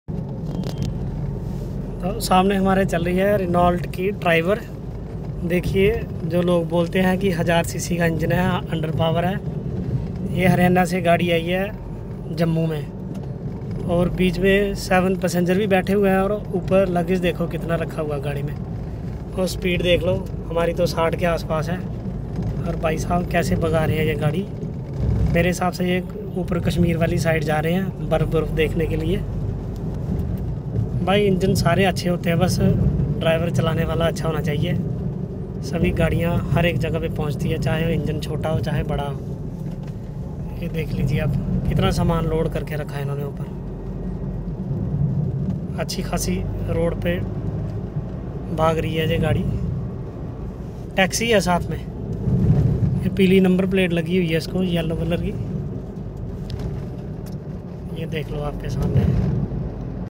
yeh vedio unke liye jo 1000cc ko underpower boltey hai
Renault triber fully 7 pessangers loaded with luggage